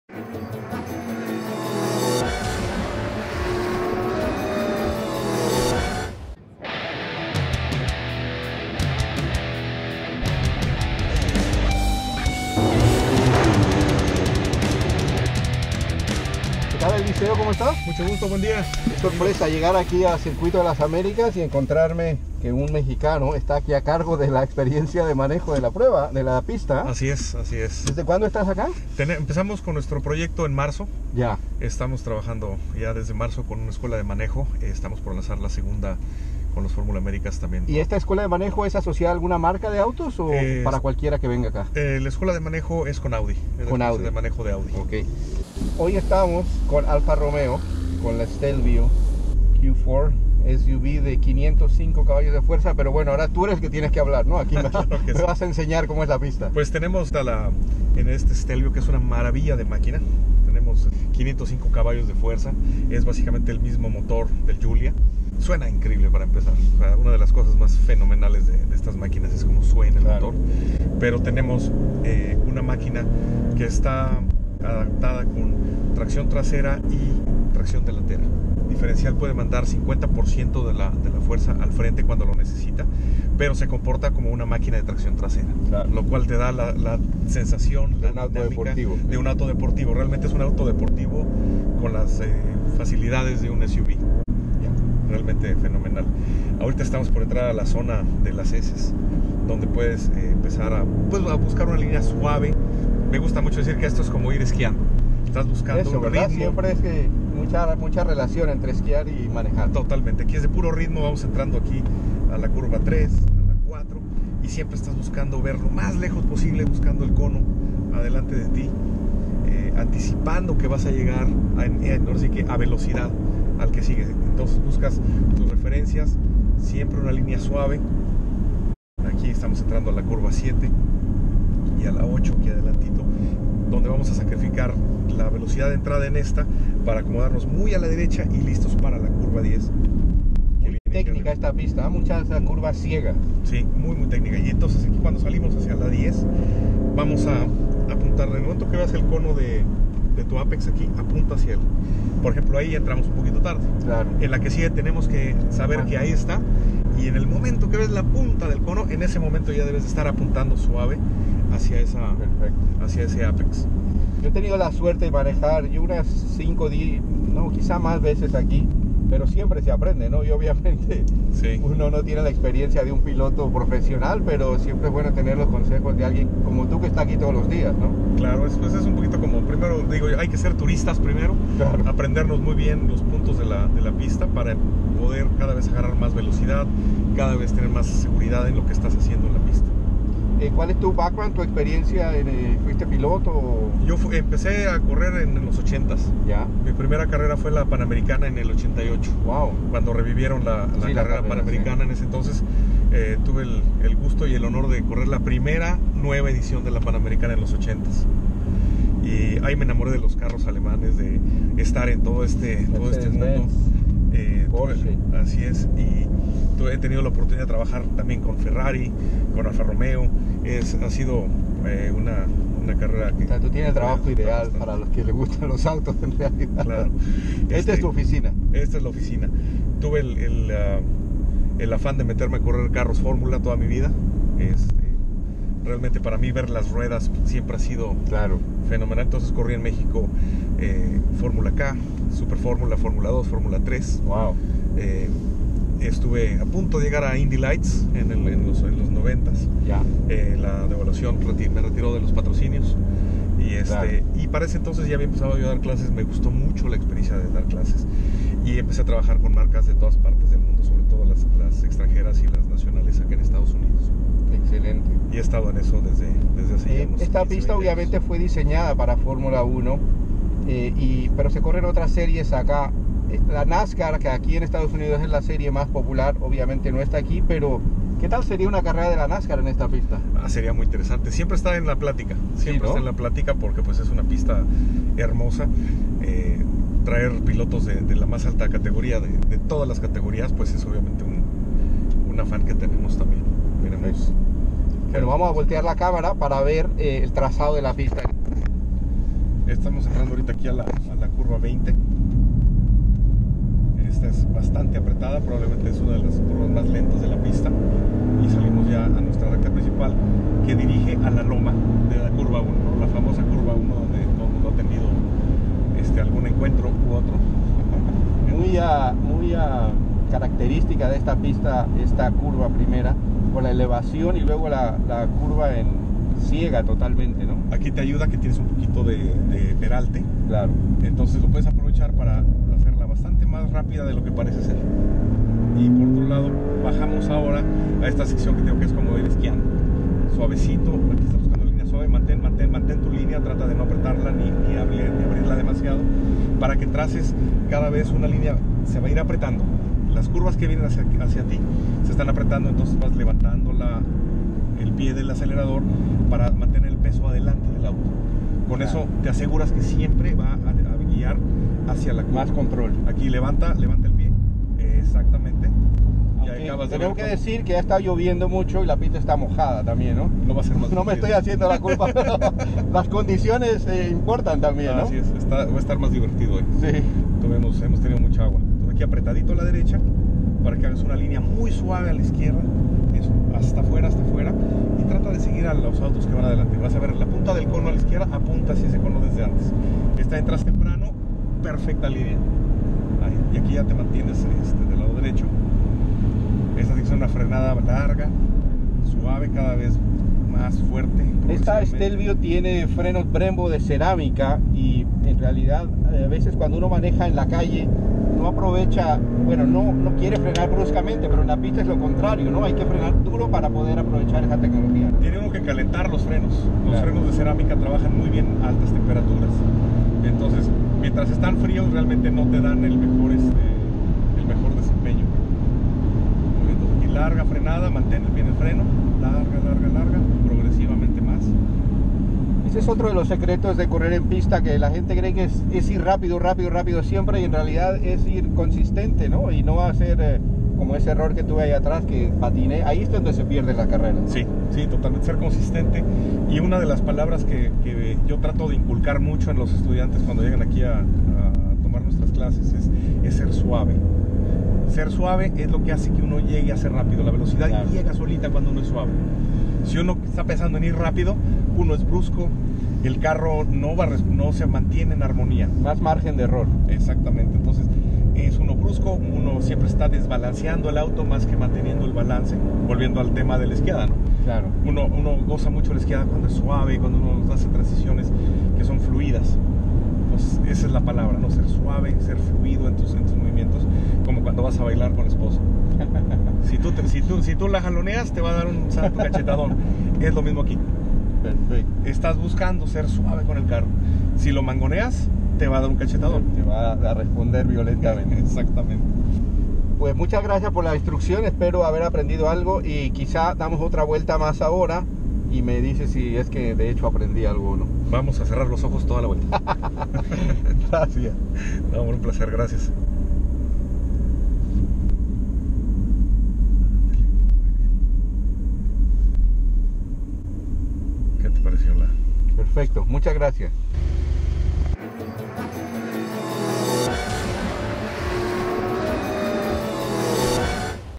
Clases de manejo con la Alfa Romeo Stelvio Quadrifoglio Q4 2018 en el Circuito de las Américas
Clases de manejo con la Alfa Romeo Stelvio Quadrifoglio Q4 2018 en el Circuito de las Américas con Eliseo Mendoza-Altamirano, el Director de la Escuela de Manejo en Pista